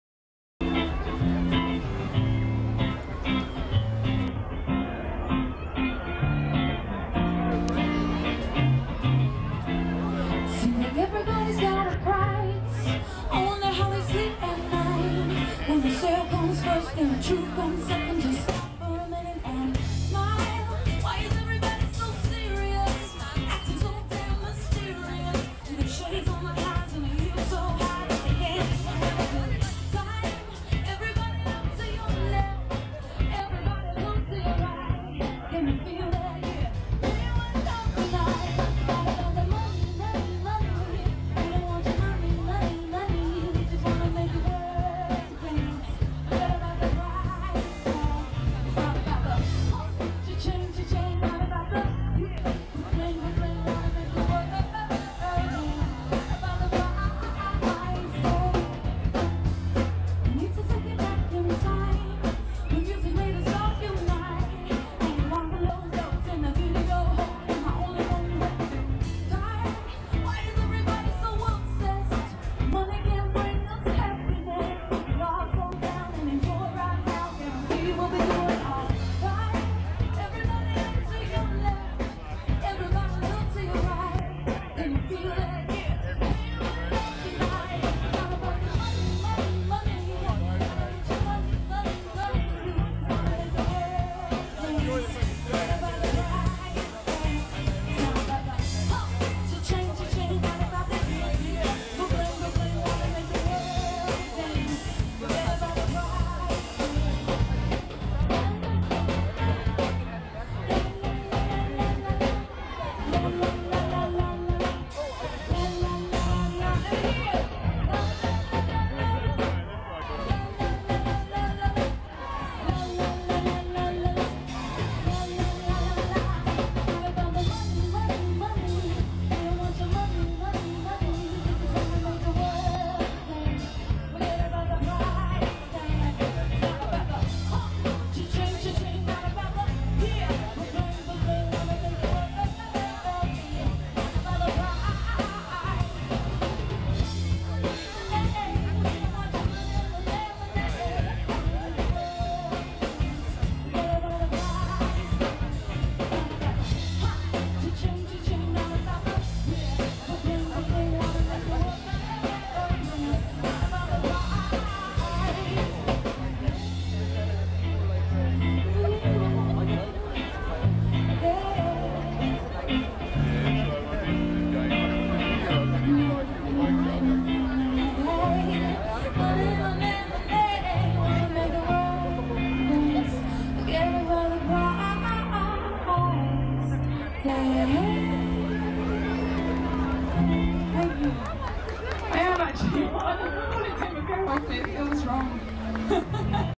DINGLEY DELL AREA HYDE PARK WINTER WONDERLAND LONDON MUSIC STAGE 2 BY COASTERS 2012
BEHIND ICE RINK GREAT BAR AREA WITH LIVE BANDS ALL FREE